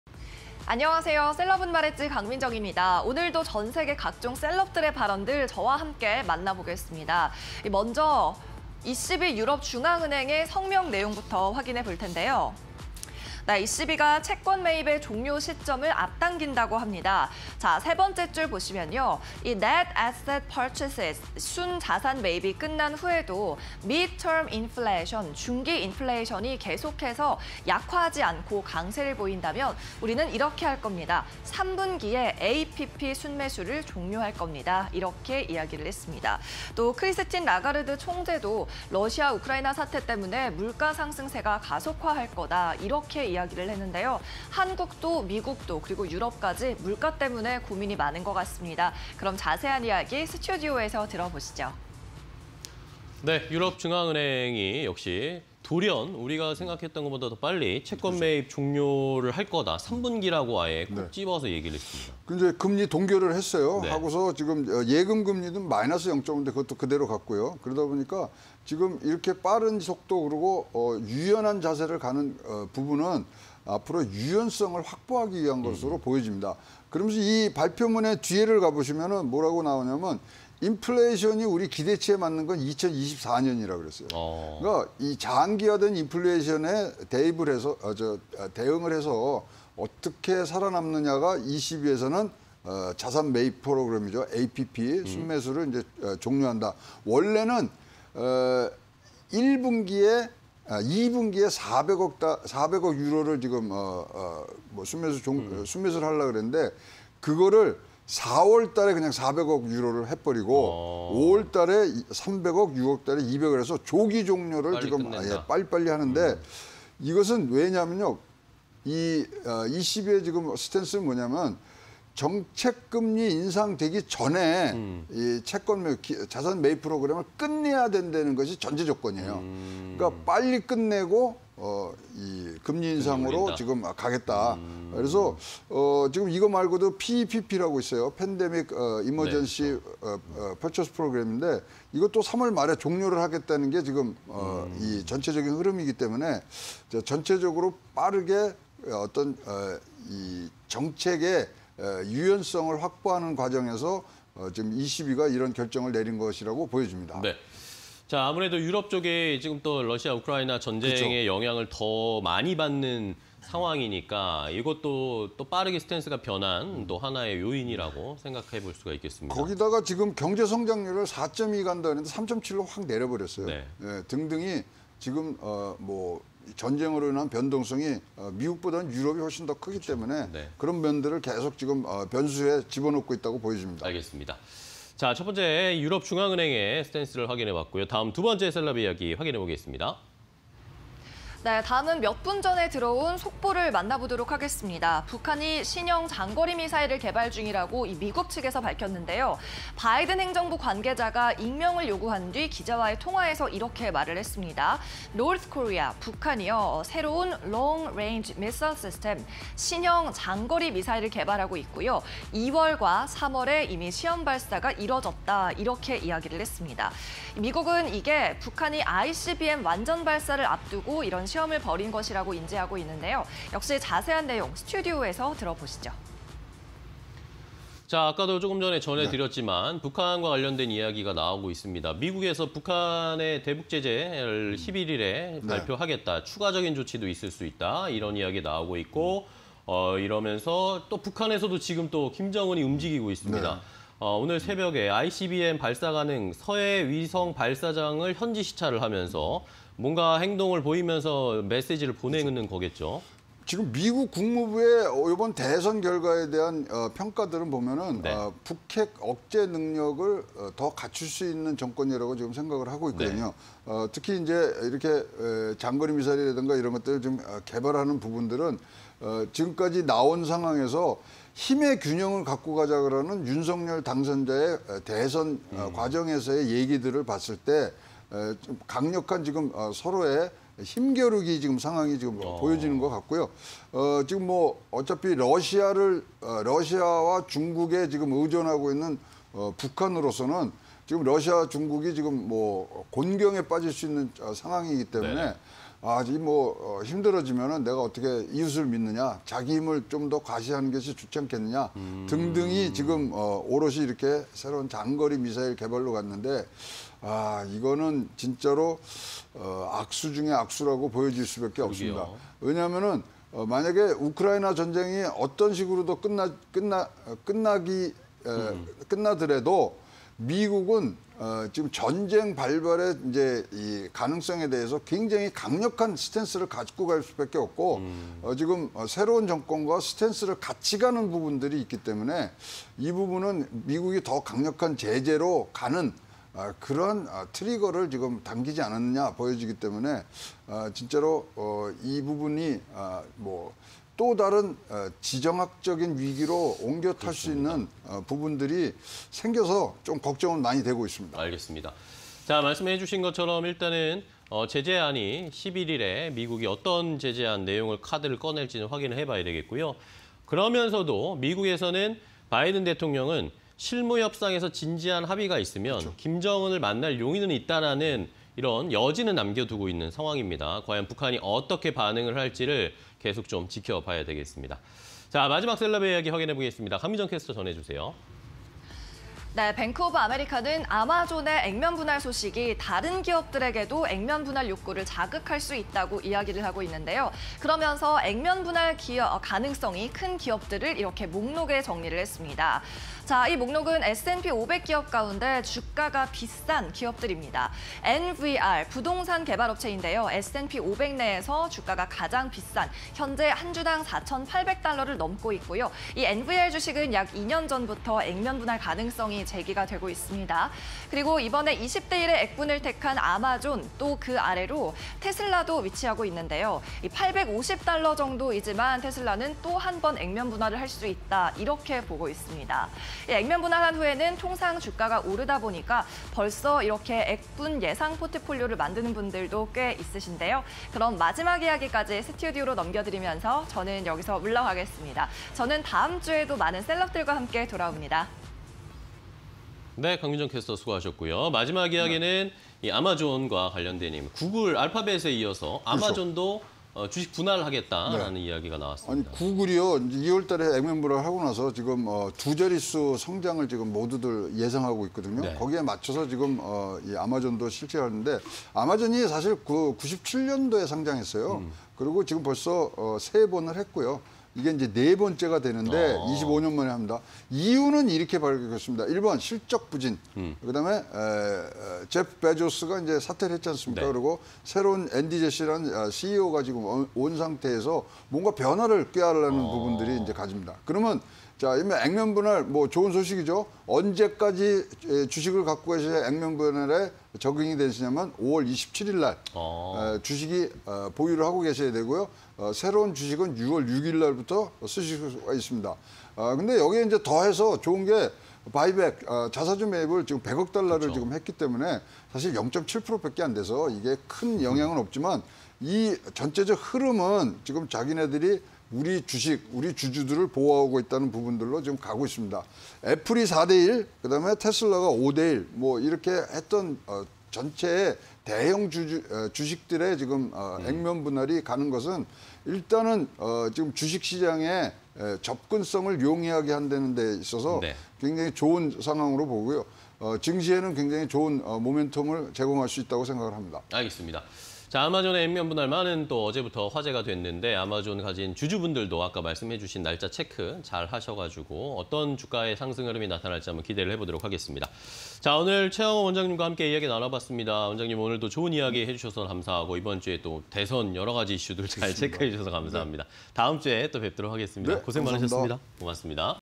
ECB 유럽중앙은행 / 익명의 바이든 행정부 관계자_셀럽은 말했지 (20220311)
ECB 유럽중앙은행 / 성명
"If the incoming data support the expectation that the medium-term inflation outlook will not weaken even after the end of our net asset purchases, the Governing Council will conclude net purchases under the APP in the third quarter."
순자산 매입이 끝난 후에도 중기 인플레이션이 약화하지 않을 것이라는 데이터가 들어온다면 3분기에 관리위원회가 APP 순매수를 종료할 것입니다.

# 익명의 바이든 행정부 관계자 / 외신 기자와의 통화
"North Korea is developing a new long-range missile system… the tests were carried out in February and March."
북한이 새로운 장거리 미사일을 개발하고 있습니다. 시험 발사는 2월과 3월에 이뤄졌습니다.